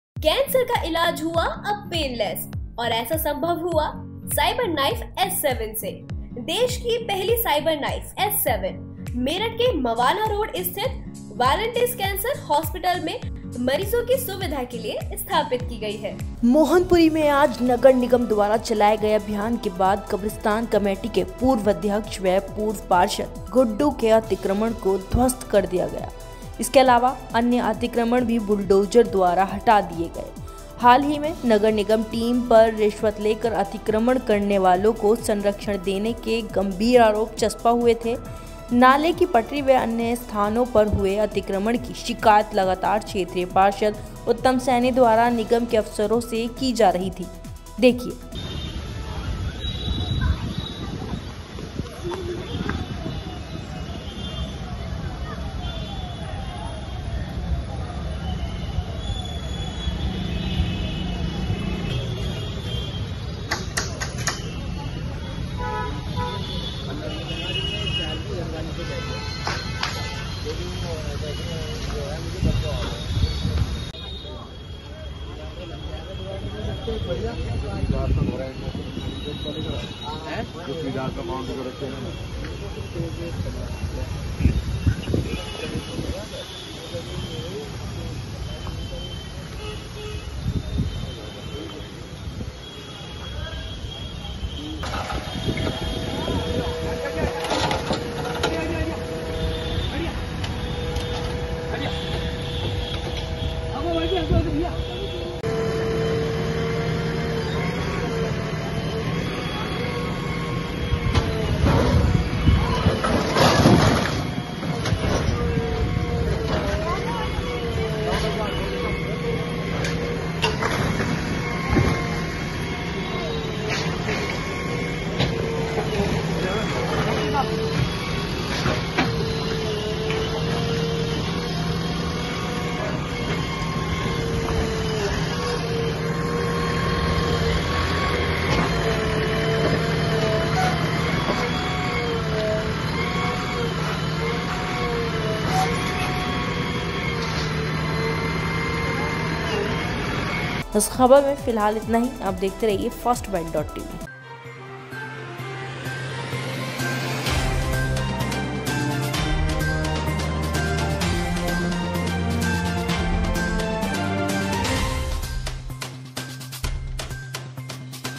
मोहनपुरी में नगर निगम ने चलाया बुलडोजर - First Byte Tv
मोहनपुरी में आज नगर निगम द्वारा चलाये गये अभियान के बाद कब्रिस्तान कमेटी के पूर्व अध्यक्ष व पूर्व पार्षद गुड्डू के अतिक्रमण को ध्वस्त कर दिया गया। इसके अलावा अन्य अतिक्रमण भी बुलडोजर द्वारा हटा दिये गये। हाल ही में नगर निगम टीम पर रिश्वत लेकर अतिक्रमण करने वालों को संरक्षण देने के गंभीर आरोप चस्पा हुए थे। नाले की पटरी व अन्य स्थानों पर हुए अतिक्रमण की शिकायत लगातार क्षेत्रीय पार्षद उत्तम सैनी द्वारा निगम के अफसरों से की जा रही थी